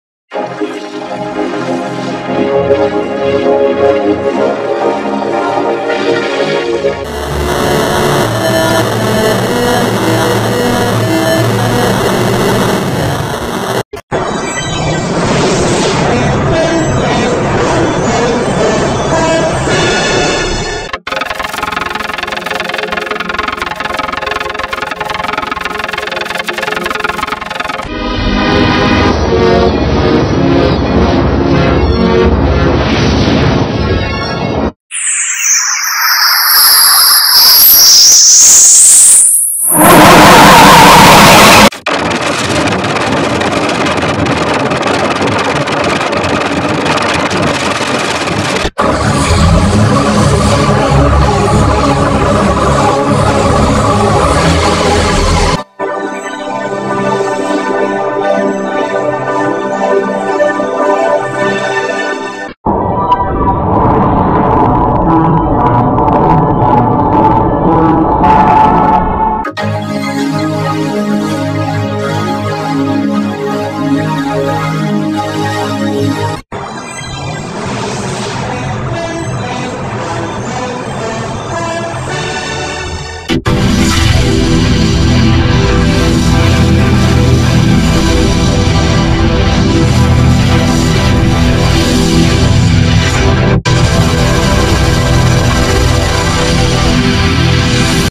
MTE 1987 Effects Round 2 Vs. Volt HD & Everyone (2/19)
Well, i don't care about Volt HD.

@Timely_1530 is next to the Round 3!

List of effects:
- CoNfUsIoN
- I Revived X
- Mystery Effect
- Caught A Cold
- Old TV
- 3.0 DUH
- CYRYUSBOI02 the object thingy's G-Major 44
- Piano Vocoder
- I Killed X
- SeaSkyFlangedSawChorded
- C Major
- YP/UCC Combo (Taken from one of my effects collab of "TheNightOfLogoMaker698 Csupo V1")

Add Me Discord Tags: sugify7381

and all the TSUGEVE7381 Logos